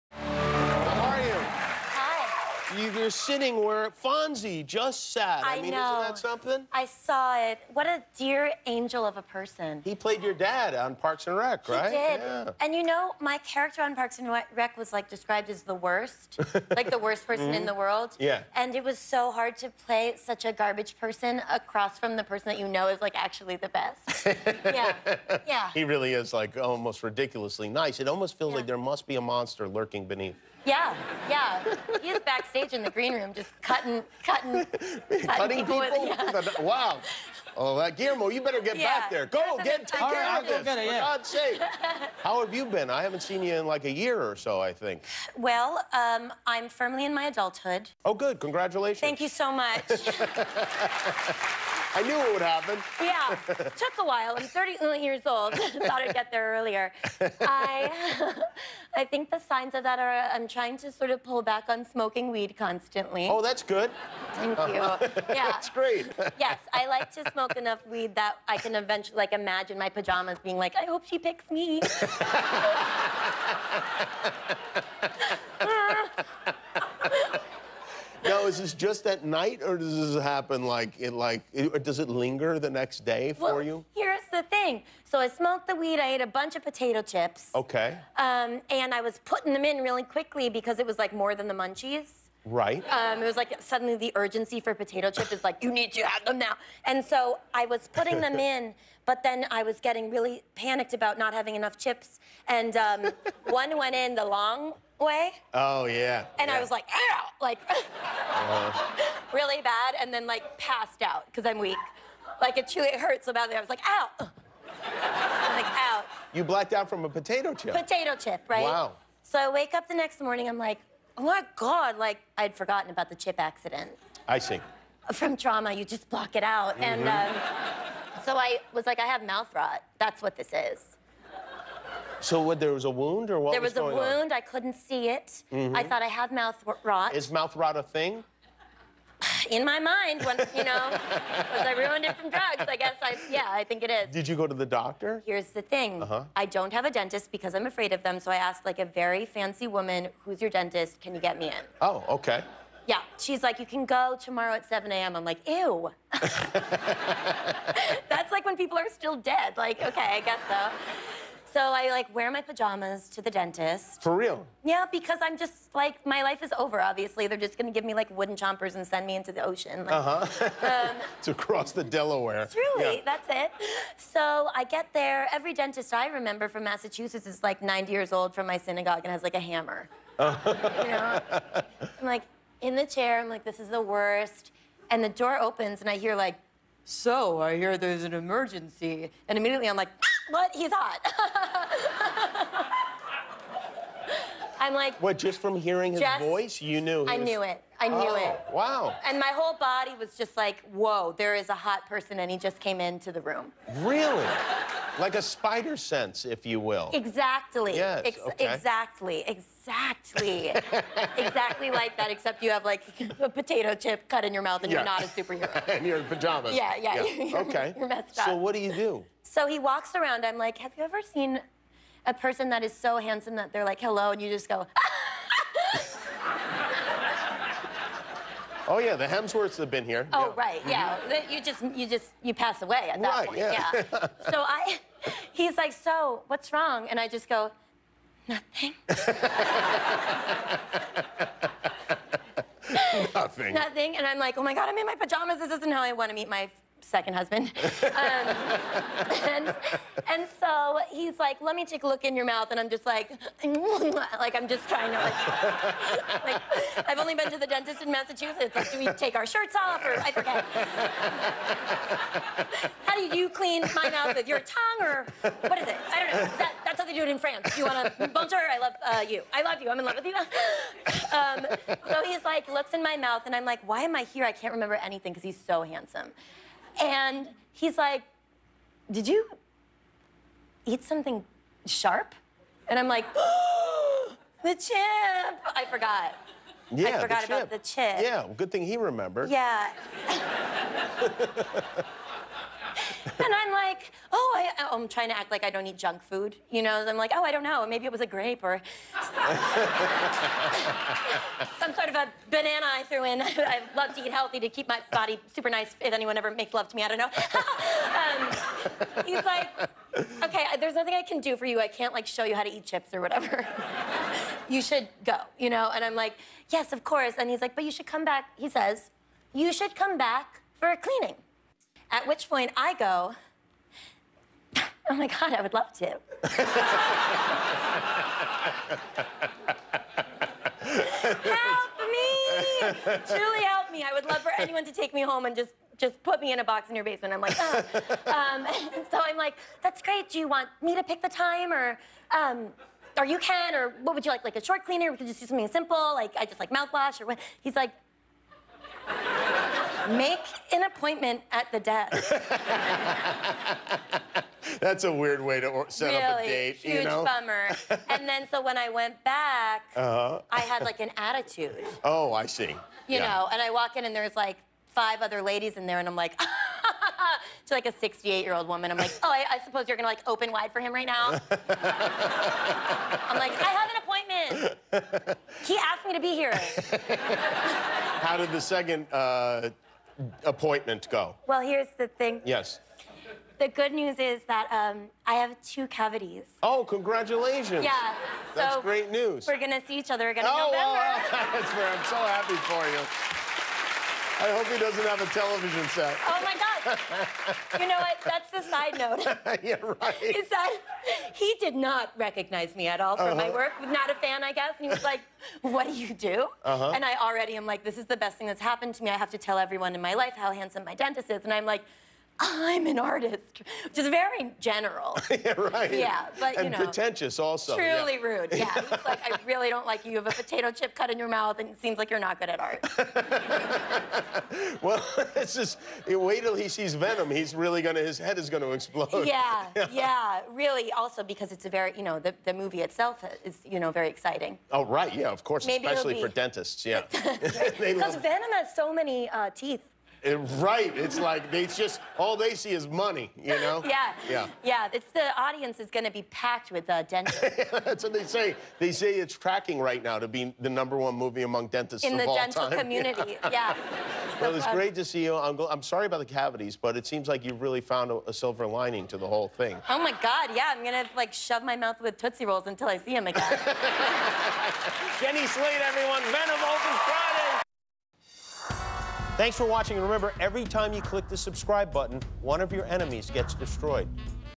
Jenny Slate's Pot Smoking Led Her to Hot Dentist
Jenny talks about working with Henry Winkler, pulling back on smoking weed, the pot-related incident that led her to fall in love with her dentist and her new movie Venom.